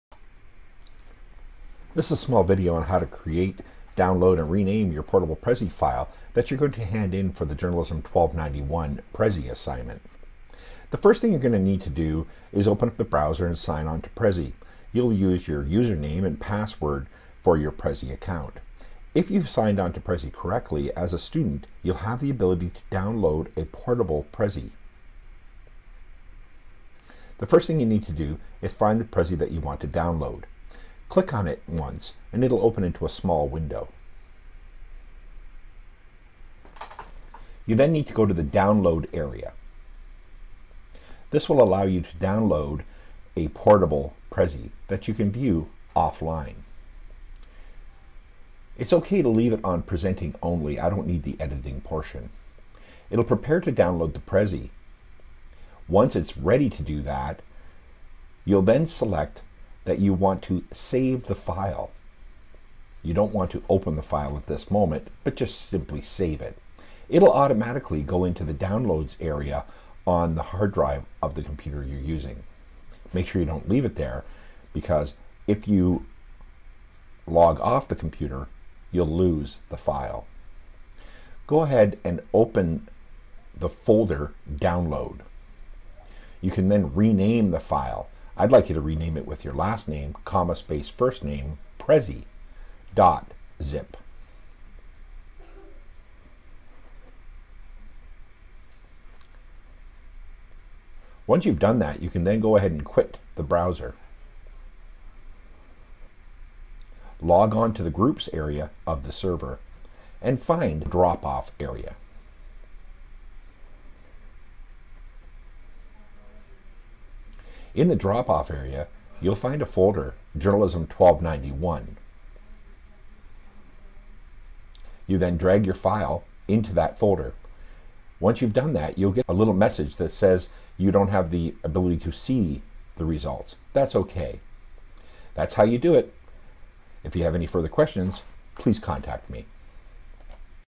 How to create a portable Prezi
How to create a portable Prezi, download, name the file and file it for marking. JOUR1291 course at Niagara College, Welland, Ontario, Canada